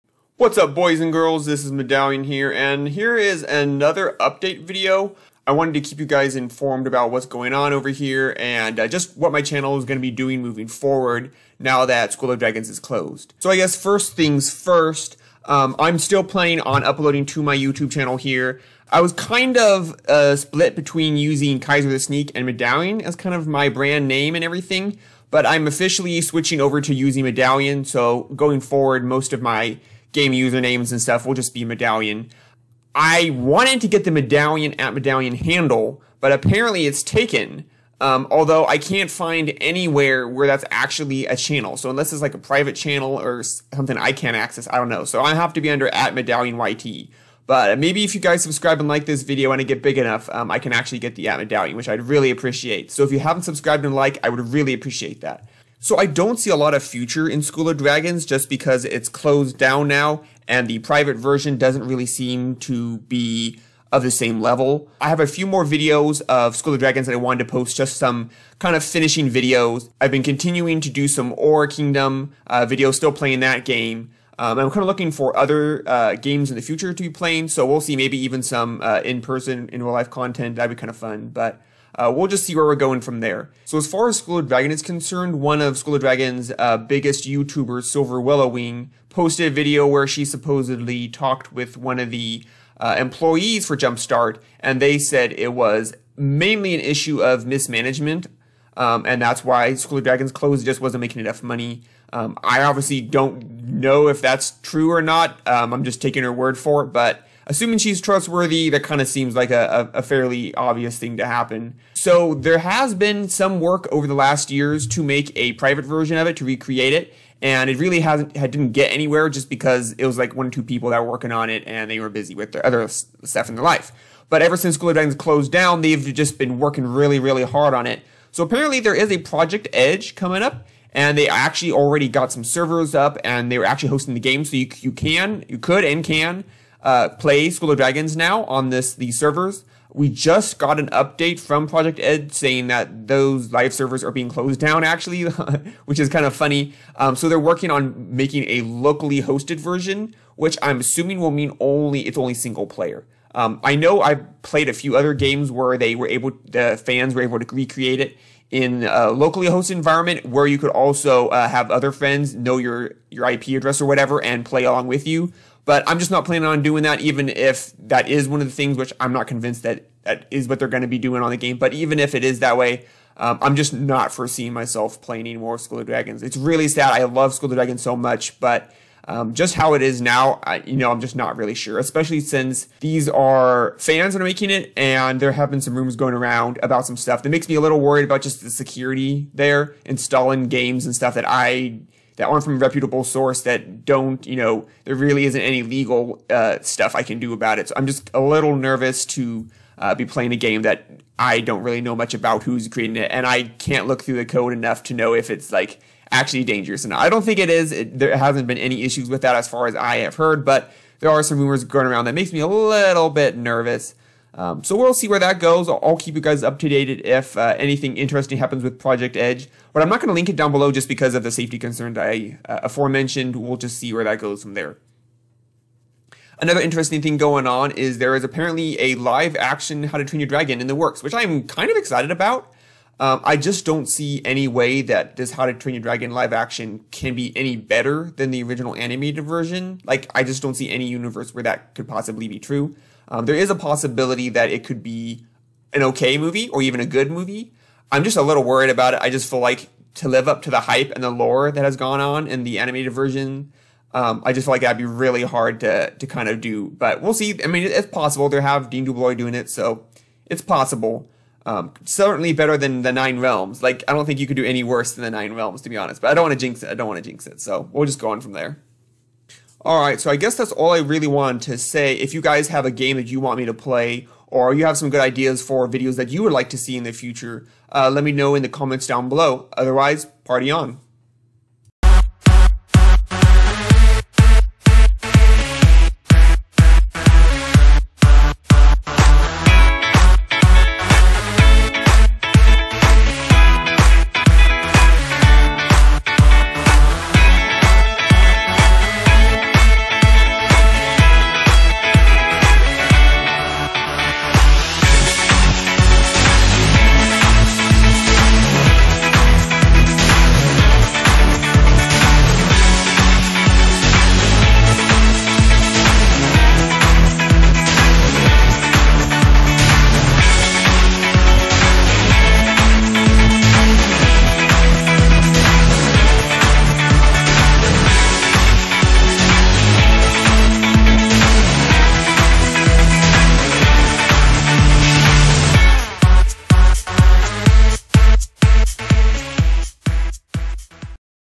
School of Dragons is Back?!
Update about my channel, School of Dragons, and the live action How To Train Your Dragon